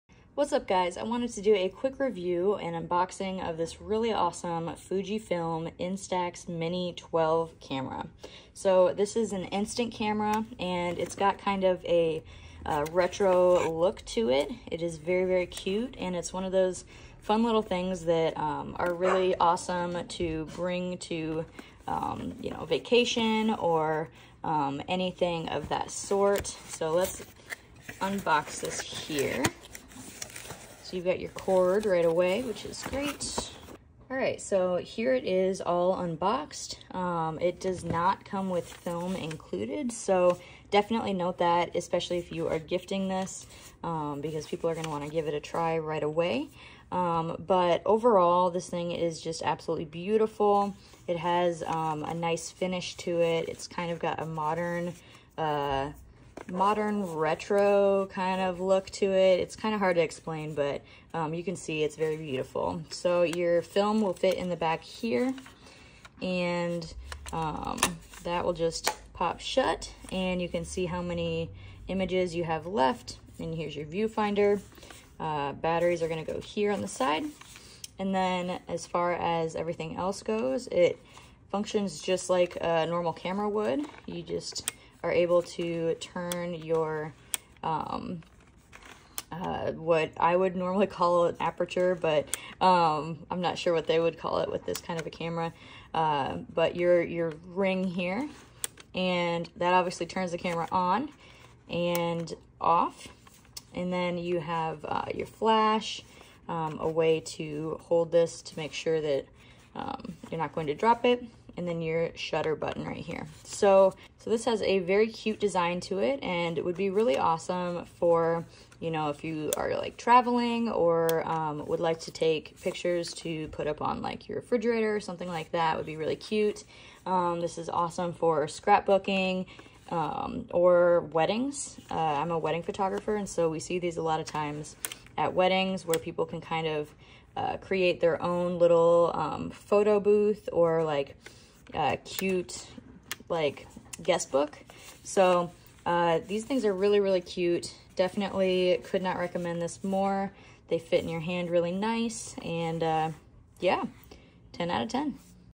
Fujifilm Instax Mini 12 Purple Holiday Bundle 2023
Fujifilm Instax Mini 12 Purple Holiday Bundle 2023 . Compact and cute design. Easily twist the lens to turn on and off
Built-in selfie mirror for easy selfies Close-up mode with parallax correction
Features automatic exposure and flash control for bright photos that are not “washed-out”
Prints out high-quality, 2” x 3” INSTAX MINI instant photos in approximately five seconds (Development time thereafter is approximately 90 seconds).
Bundle Includes: 10pk Instax Mini instant print film, Instax Mini Photo Album - holds 32 Instax Mini photos, Instax "Love" Photo Holder with magnet - holds 1 Instax photo, and Instax Photo Idea Booklet outlining creative projects that can be created using printed Instax photos. Also Included: Hand strap, user manual, (2) AA batteries and 1-year limited warranty